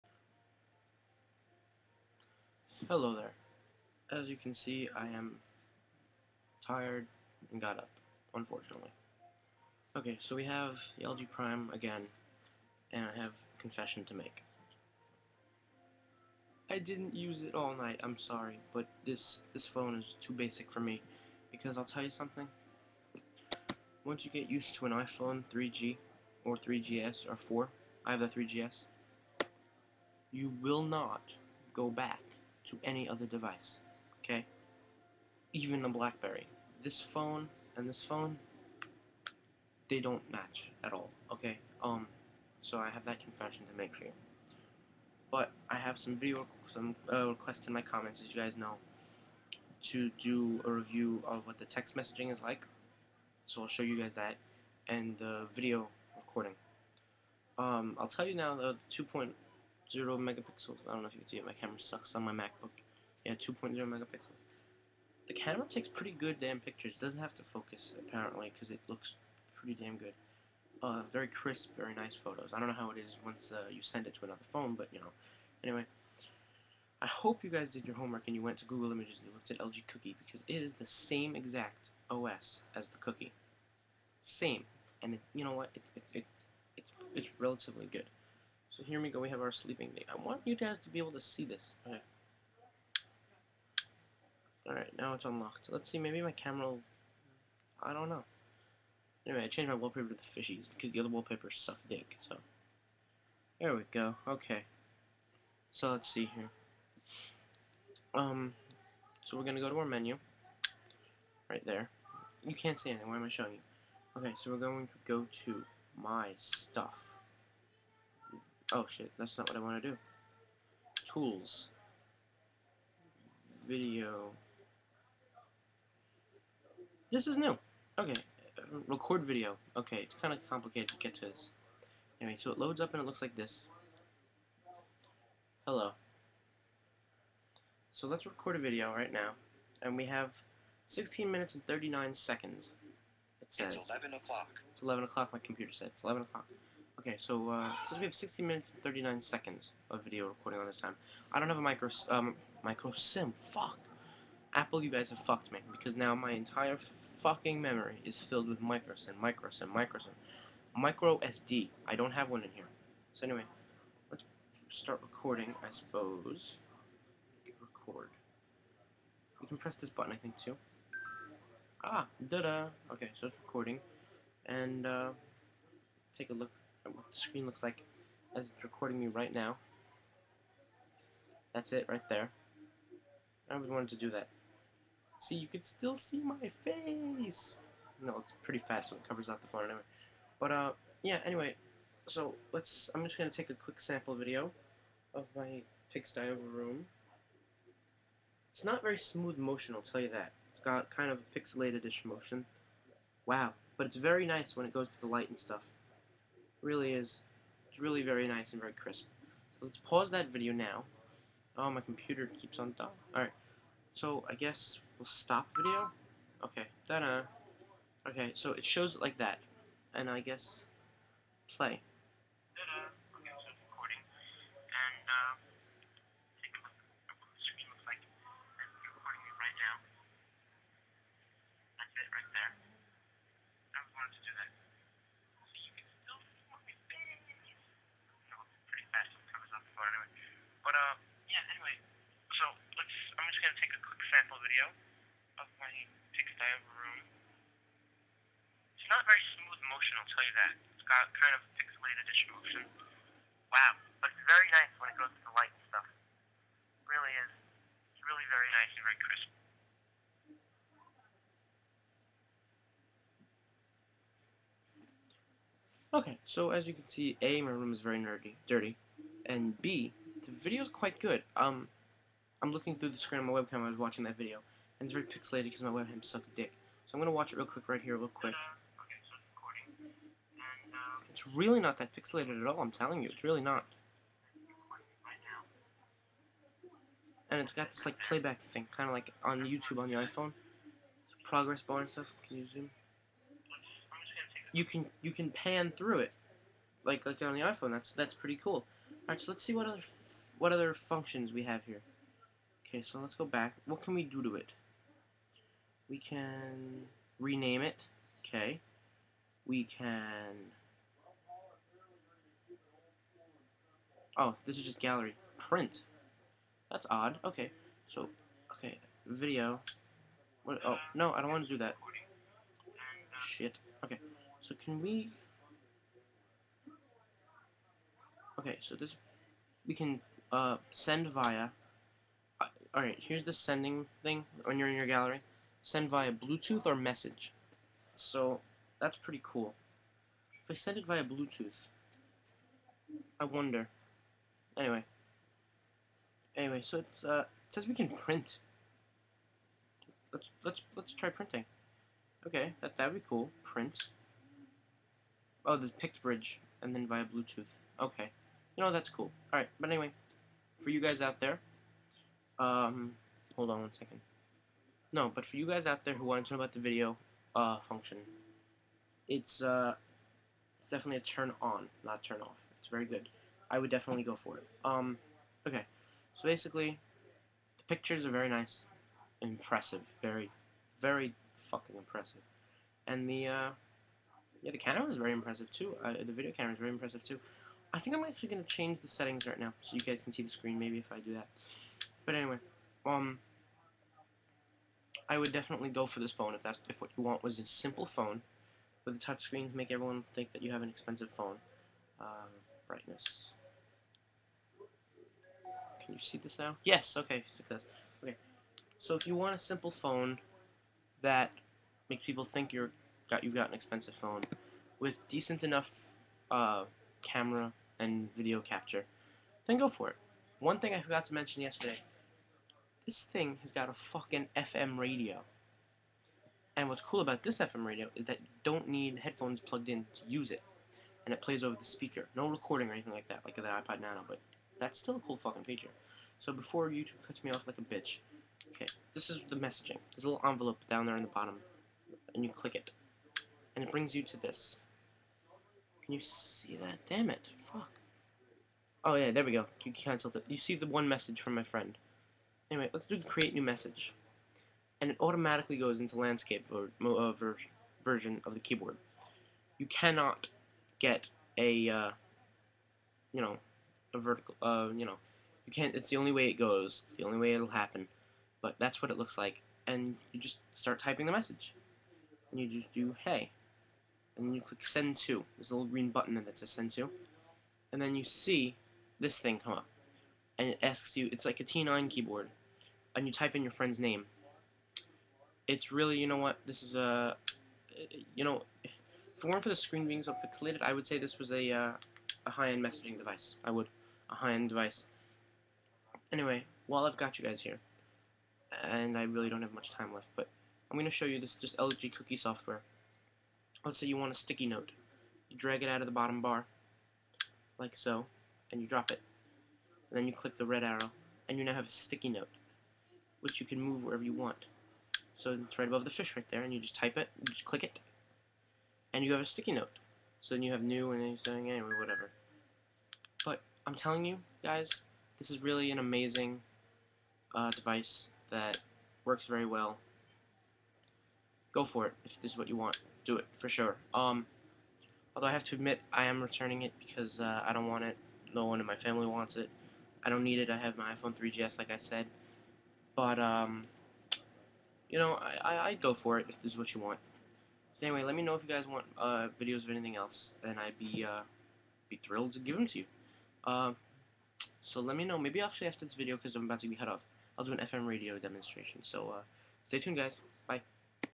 LG Prime Messaging and Video Camera
this is just a very quick and simple review and overview of the lg primes text messaging and video camera